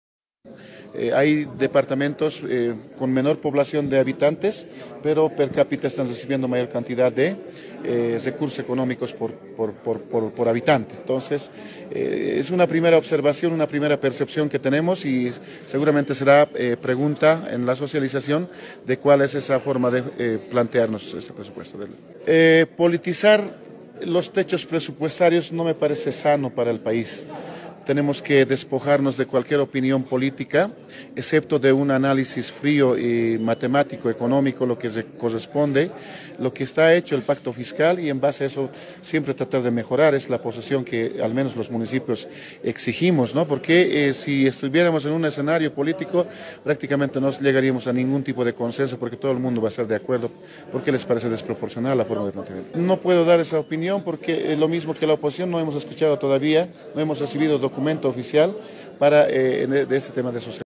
AGAMDEPAZ SEÑALA QUE NO SE DEBE POLITIZAR LA APROBACION DEL PRESUPUESTO GENERAL DEL ESTADO 2024
🔻LaPaz
El Presidente de AGAMDEPAZ, Napoleon Yahuasi, señalo que no se debe politizar el proyecto de ley 030/23-24 del Presupuesto General del estado (PGE) 2024